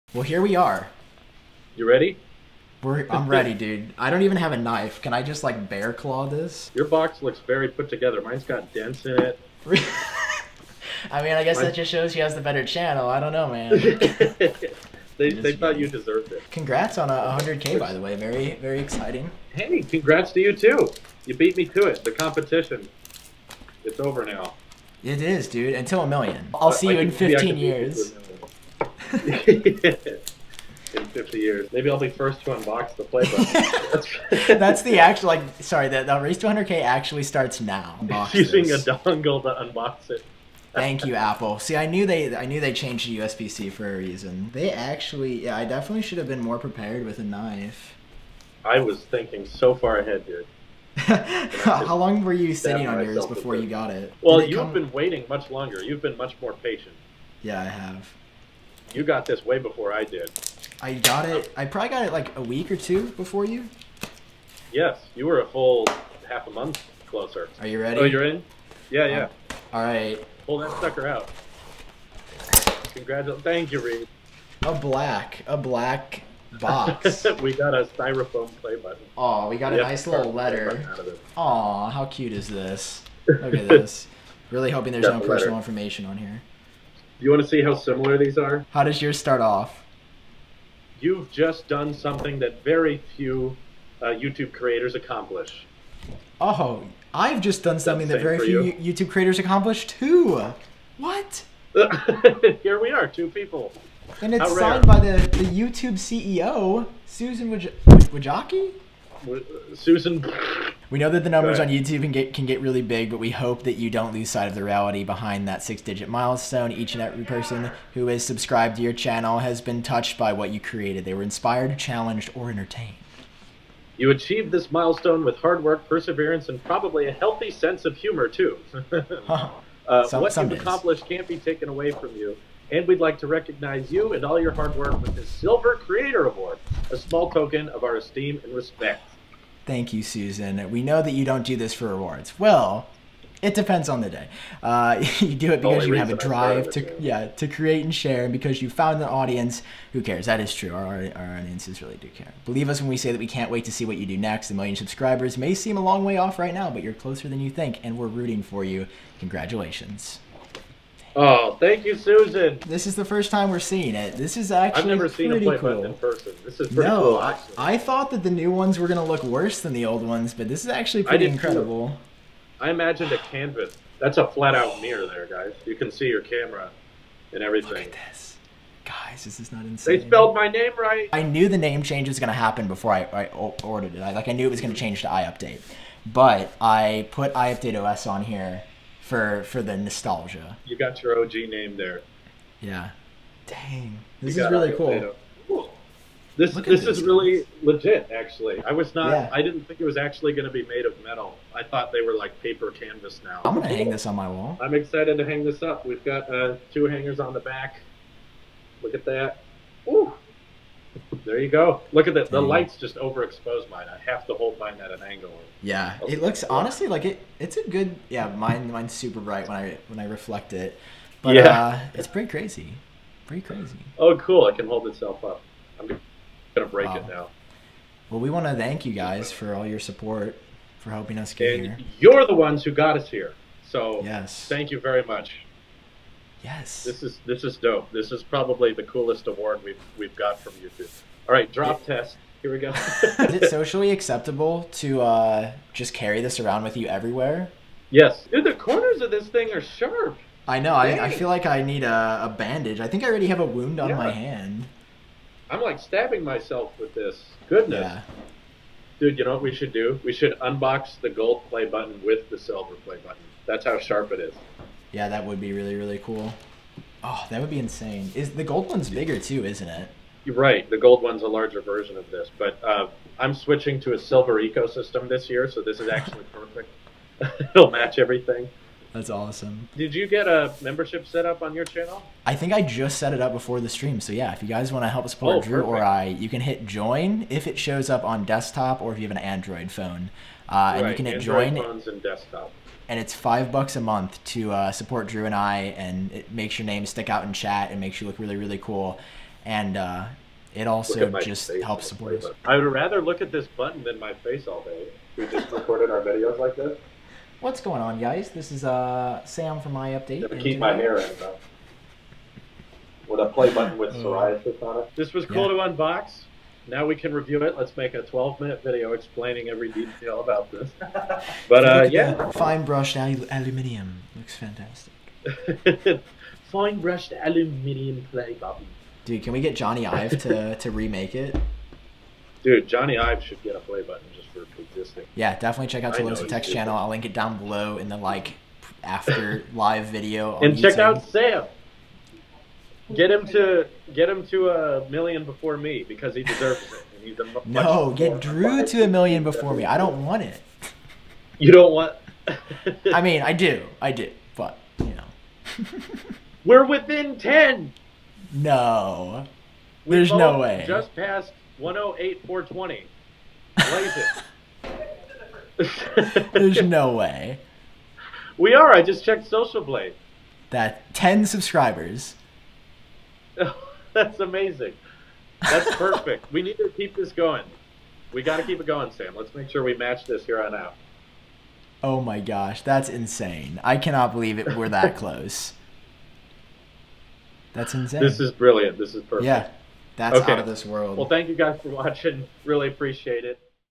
2x YouTube Play Button Unboxing! (feat. Tailosive Tech)
Drew (Tailosive Tech) and I unbox our silver play buttons for surpassing 100,000 subscribers on YouTube. We couldn't have gotten here without your support, so thank you!